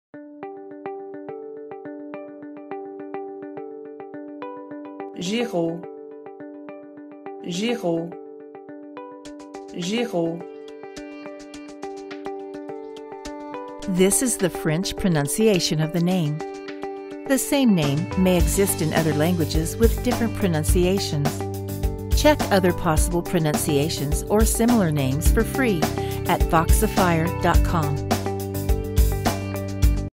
How to Pronunce Giraud in French - Voxifier.com
This is the French pronunciation, popularity, and phonetics recording of the surname Giraud. The same name may exist in other languages with different pronunciations.